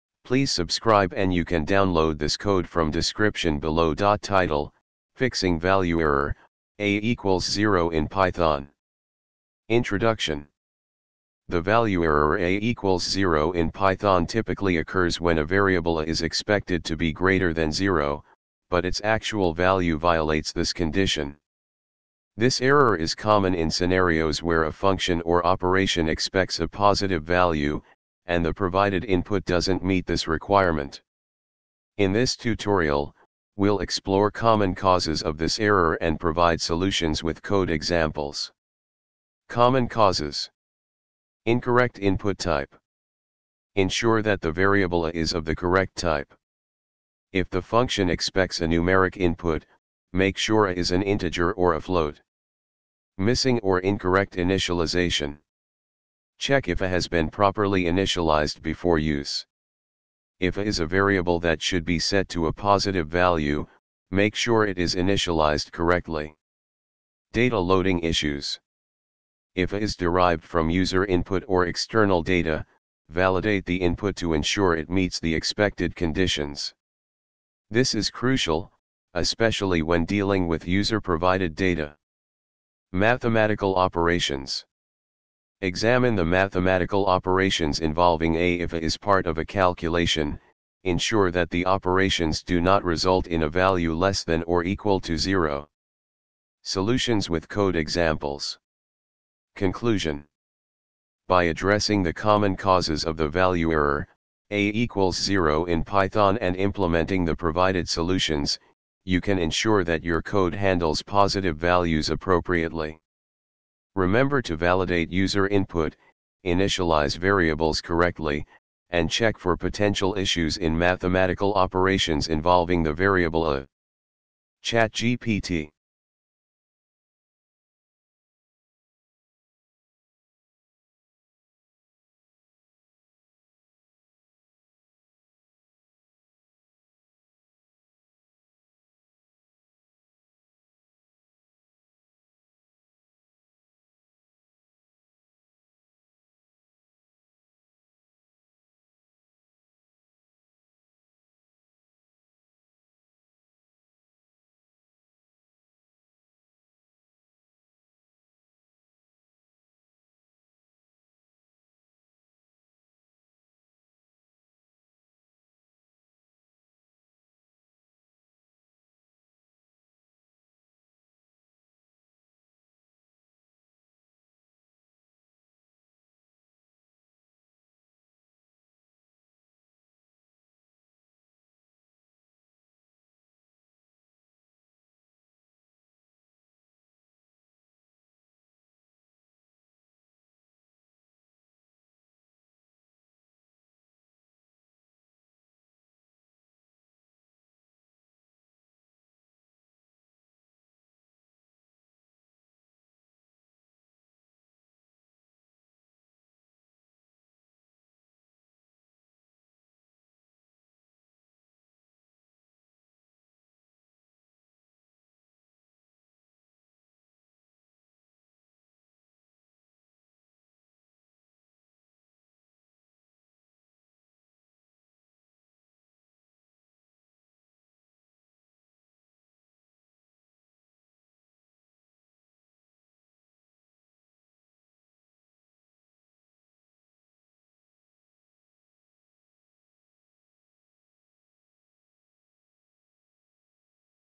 how to fix ValueError a 0 in python
Title: Fixing ValueError: a = 0 in Python
Introduction:
The ValueError "a = 0" in Python typically occurs when a variable 'a' is expected to be greater than zero, but its actual value violates this condition. This error is common in scenarios where a function or operation expects a positive value, and the provided input doesn't meet this requirement. In this tutorial, we'll explore common causes of this error and provide solutions with code examples.
Common Causes:
Incorrect Input Type:
Ensure that the variable 'a' is of the correct type. If the function expects a numeric input, make sure 'a' is an integer or a float.
Missing or Incorrect Initialization:
Check if 'a' has been properly initialized before use. If 'a' is a variable that should be set to a positive value, make sure it is initialized correctly.
Data Loading Issues:
If 'a' is derived from user input or external data, validate the input to ensure it meets the expected conditions. This is crucial, especially when dealing with user-provided data.
Mathematical Operations:
Examine the mathematical operations involving 'a.' If 'a' is part of a calculation, ensure that the operations do not result in a value less than or equal to zero.
Solutions with Code Examples:
Conclusion:
By addressing the common causes of the "ValueError: a = 0" in Python and implementing the provided solutions, you can ensure that your code handles positive values appropriately. Remember to validate user input, initialize variables correctly, and check for potential issues in mathematical operations involving the variable 'a'.
ChatGPT